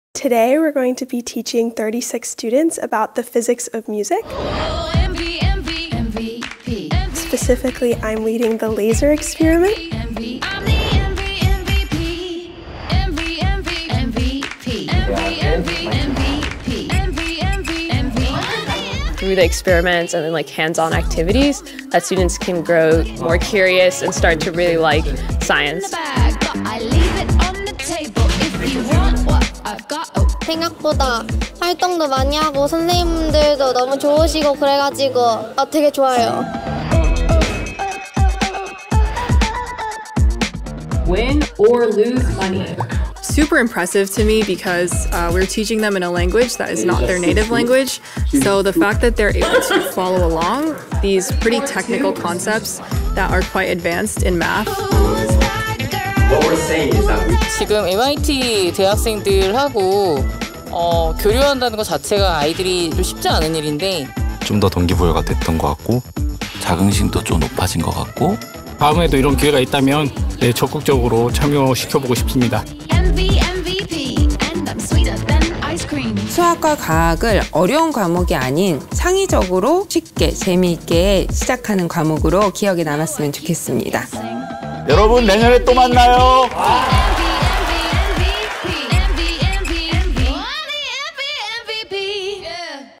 [YBM교과서] 2024 Math&Science Camp with MIT students 현장 스케치(Korean ver.)
2024년 1월! 함께해서 소중했던 추억으로!

2024년 1월 24일~25일, 이틀에 걸쳐
MIT학생들이 수업을 기획, 구성하고 선생님으로 함께 한
수학&과학 캠프가 개최됐습니다!🤓🎉
캠프의 뜨거운 현장, 여러분도 함께 즐겨보실까요?